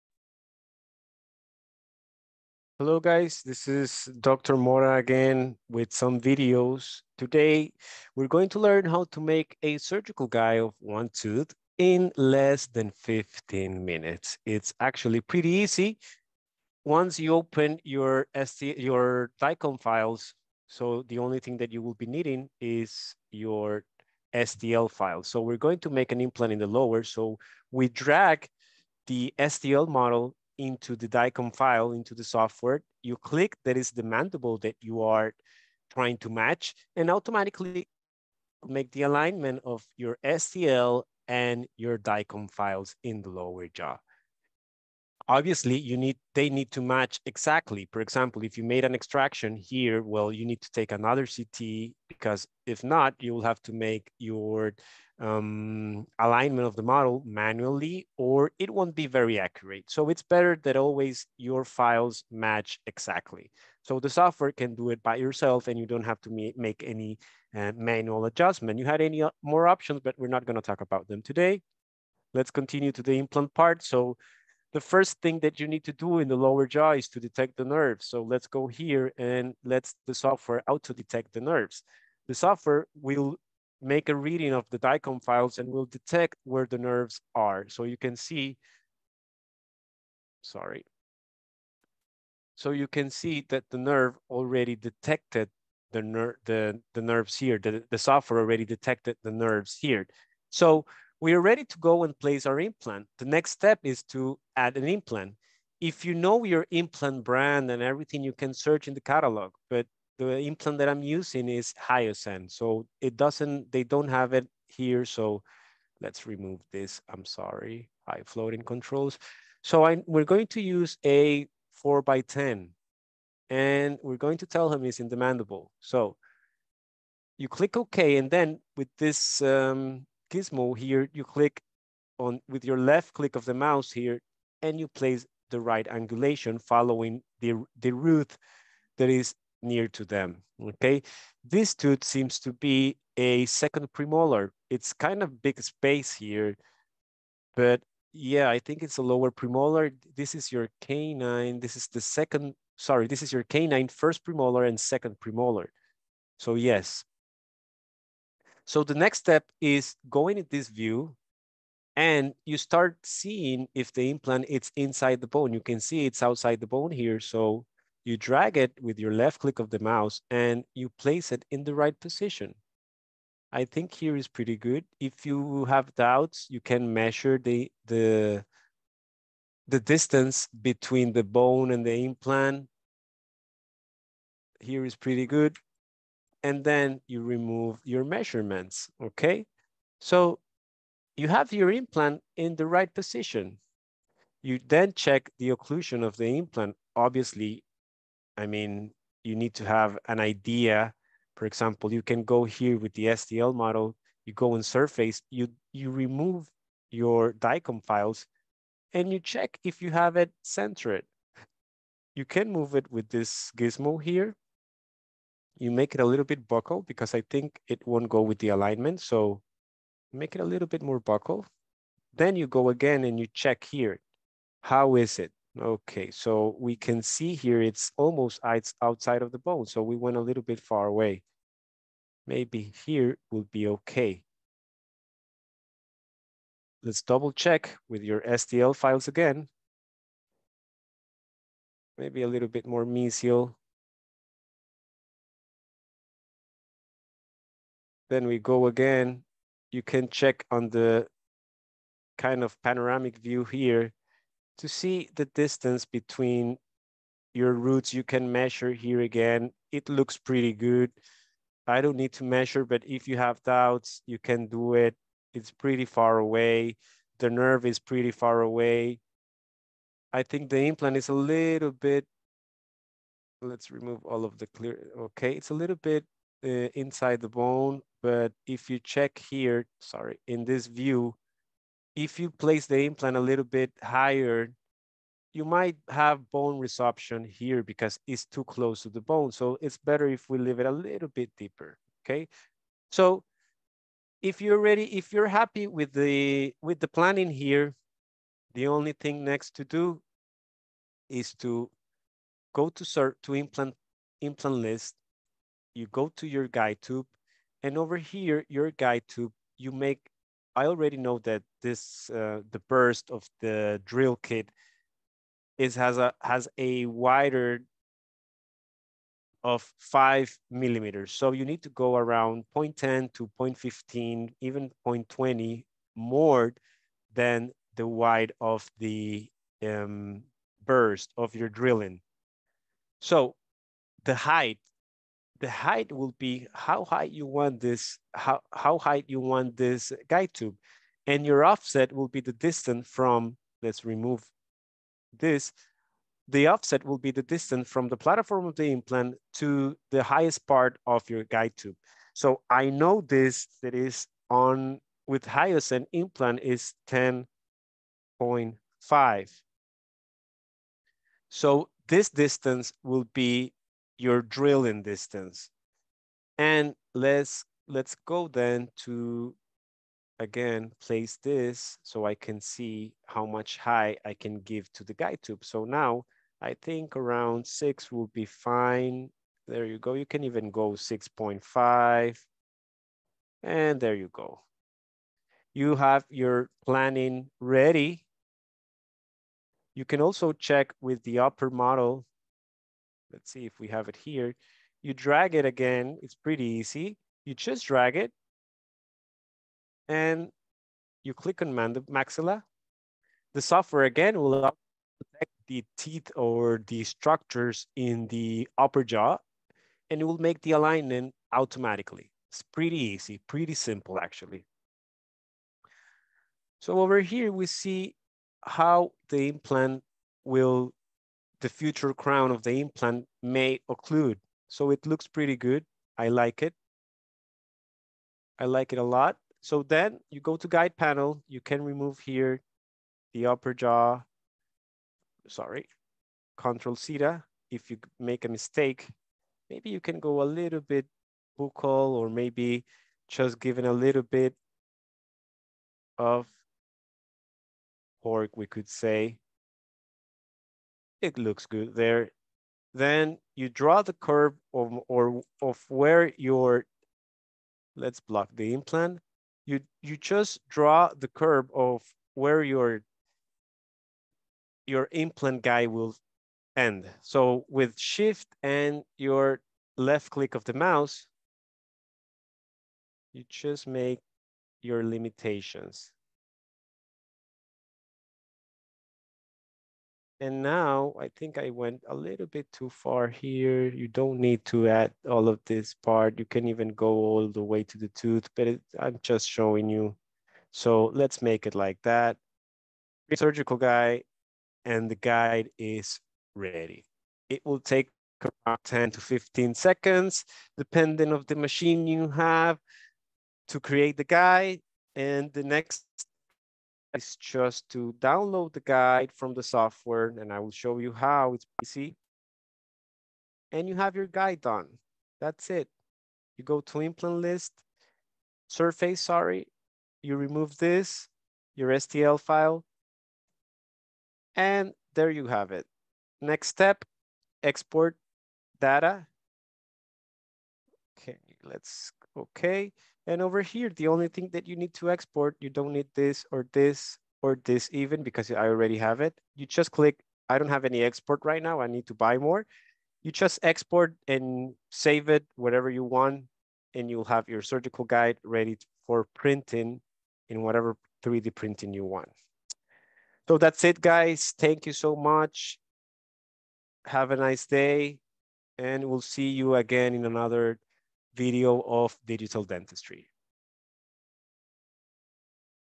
Implant Surgical Guide in less than 15 minutes with blue sky bio
How to performed a Surgical guide for dental implantes in less than 15 minutes with software blue sky bio.